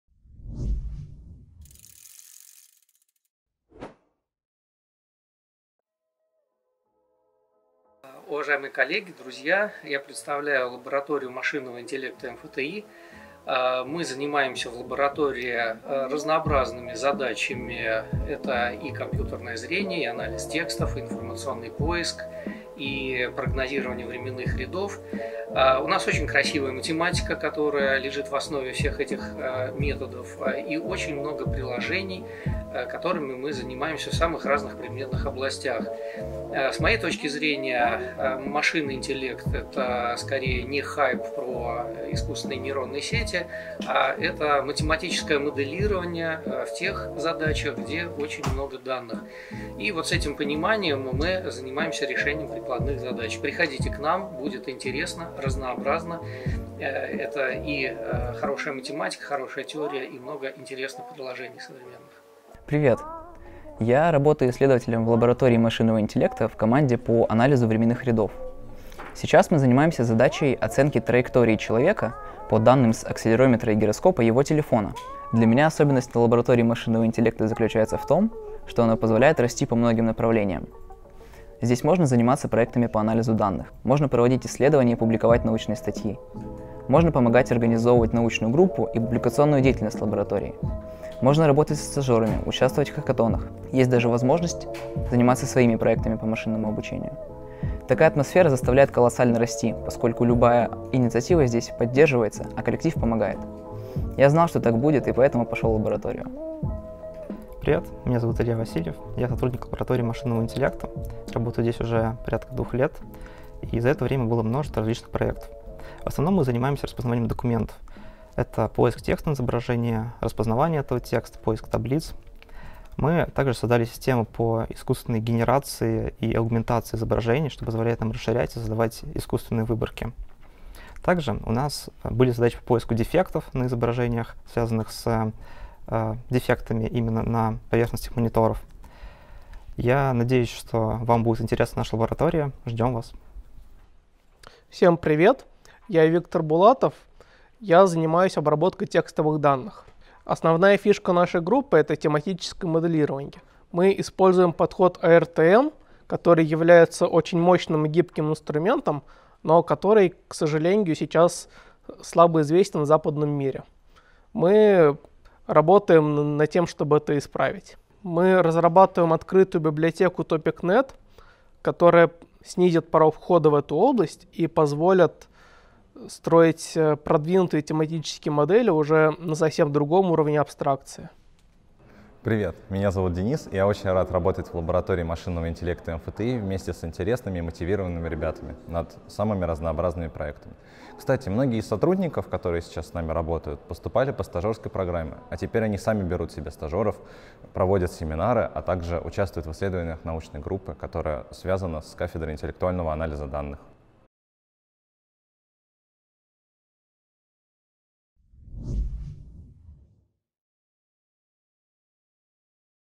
Наши сотрудники о лаборатории машинного интеллекта МФТИ
Сотрудники лаборатории mipt.ai рассказывают о сферах исследований в лаборатории машинного интеллекта МФТИ.

0:00 Константин Воронцов об основных направлениях
1:01 Тамаз Гадаев про задачу оценки траектории человека (анализ временных рядов - Timeseries Analysis)
1:46 Илья Васильев о проекте по распознаванию документов и поиске дефектов (OCR)
2:23 Виктор Булатов про задачи обработки текстовых данных и нашу open-source библиотеку TopicNET.
3:01 Денис Айвазов о стажировке и научной группе лаборатории